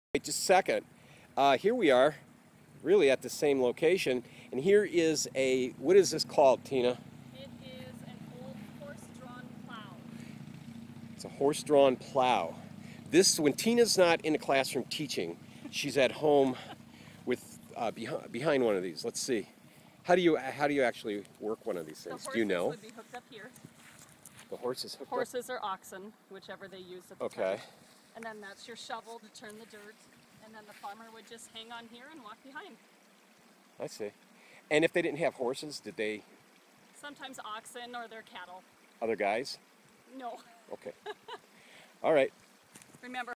Horse drawn Plough
Farm equipment like this helped convert the prairie into great farmland. Tina and her husband have a 3,500 acre farm. I have learned a lot about farming in ND from her. Farming is one of the reasons that ND is such a successful state and great place to live!